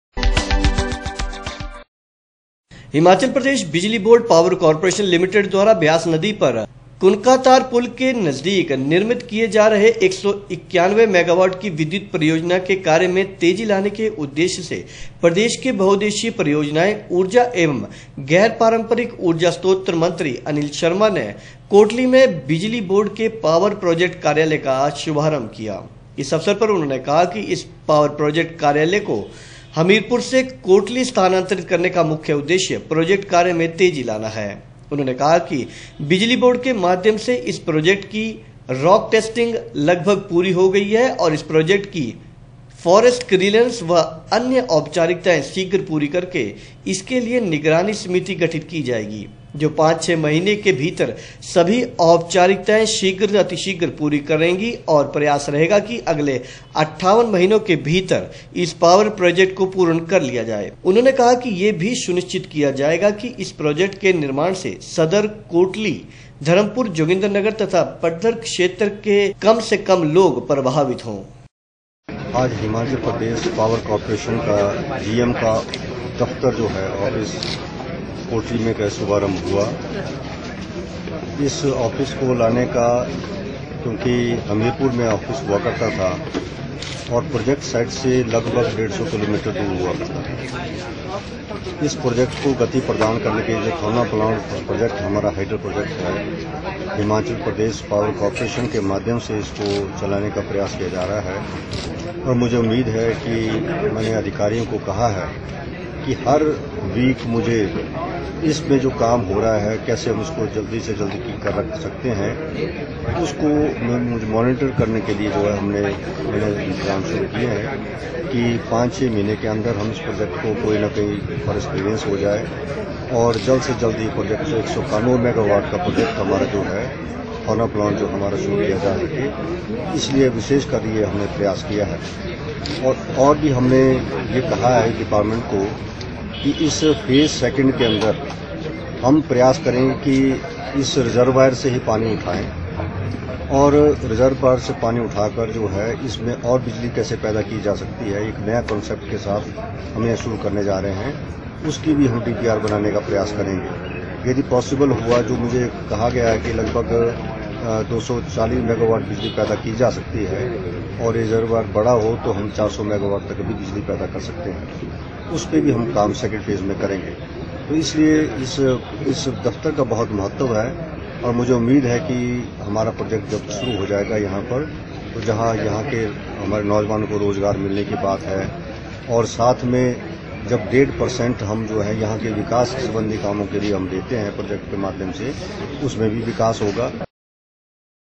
परियोजना के कार्य में तेजी लाने के उददेश्य से प्रदेश के बहुददेशीय परियोजनाएं,
हिमाचल प्रदेश बिजली बोर्ड पावर काॅरपोरेशन लिमिटिड द्वारा व्यास नदी पर कुनकातर पुल के नजदीक निर्मित किए जा रहे 191 मैगावाट की विद्युत परियोजना के कार्य में तेजी लाने के उददेश्य से प्रदेश के बहुददेशीय परियोजनाएं, ऊर्जा एवं गैर पारम्परिक ऊर्जा स्रोत मन्त्री अनिल शर्मा ने आज कोटली में बिजली बोर्ड के पावर प्रोजेक्ट कार्यालय का शुभारम्भ किया